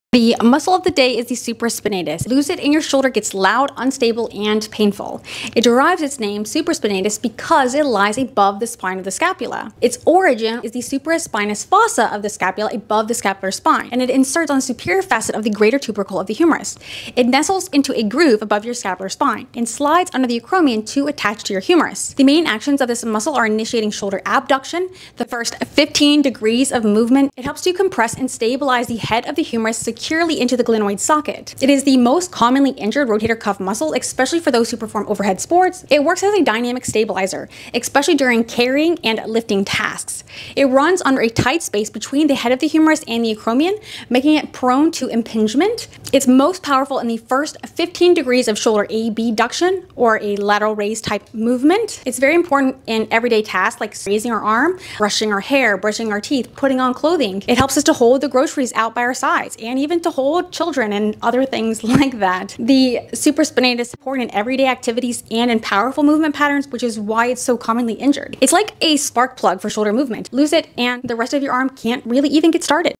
SUPRAPINATUS: THE SHOULDER STARTER!
MUSCLE OF THE DAY. THE SUPRASPINATUS, MOST COMMON INJURED ROTATOR CUFF MUSCLE. LEARN THIS TO TAKE BETTER CARE OF YOUR ROTATOR CUFF AND KEEP YOURSELF FROM BEING A STATISTIC.